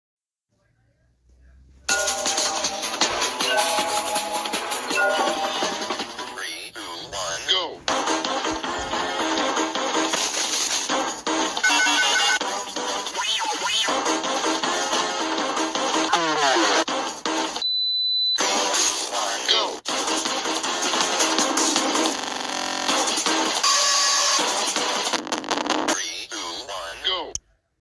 3 2 1 go turbus parte 2
este video se trata un turbus el antes del accidente y después en accidente es un meme de un youtuber que lo iso y ami se me ocurrió es de turbus y esta es la parte 2 y bye